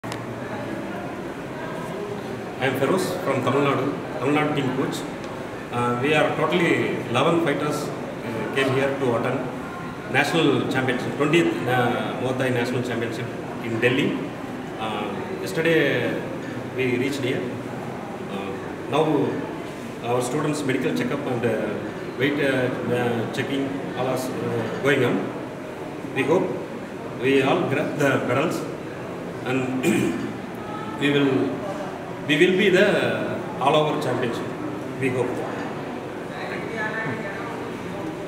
Tamil Nadu Team Interview 20th Senior National amateur MuayThai championship 2019 8th to 11th Sep
20th Senior National Muay Thai amateur championship 2019

Muaythaiboxingacademy_sadiqnagar New Delhi

Rajdhani Association of Muay Thai(Regd.) Organising the 20th Senior National Muay Thai amateur championship 2019 from 8th to 11th September, under the banner of Muay Thai India(Regd.)

Videos and pictures by Abhishek Singh @ab_photography_hub